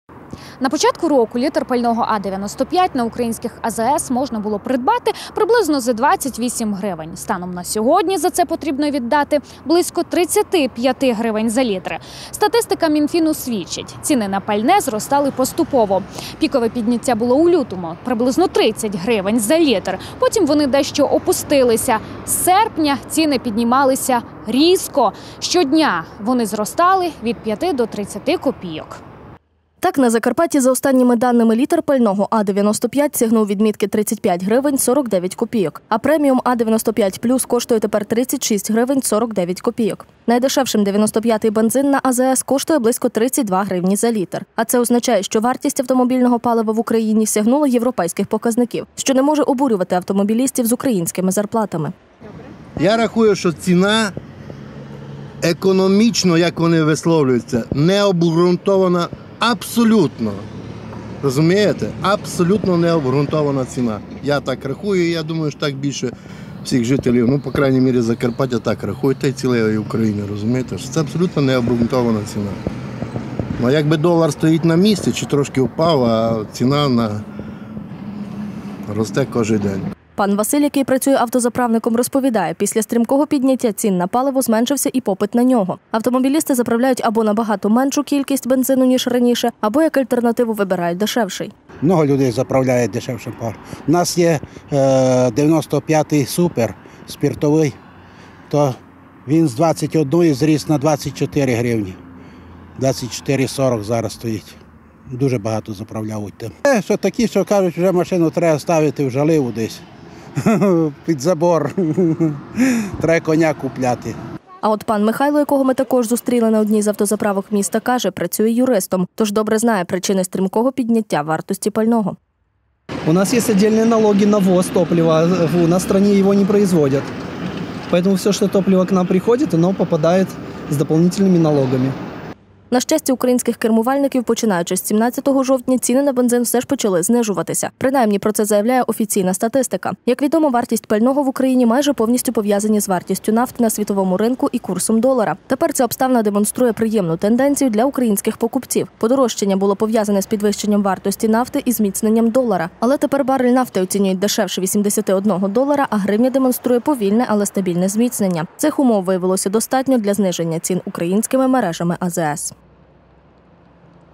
Бензин «по-європейськи»
Жовтень підготував неприємний сюрприз для українських водіїв. Ціни на пальне різко підскочили вгору і досягли рекордних показників. Цифри на табло автозаправок змінювалися чи не щодня. Стрімке здорожчання вартості бензину та дизпалива викликало обурення українських автовласників. Як коментують ситуацію закарпатські кермувальники та чи слід чекати найближчим часом зниження вартості пального – у сюжеті.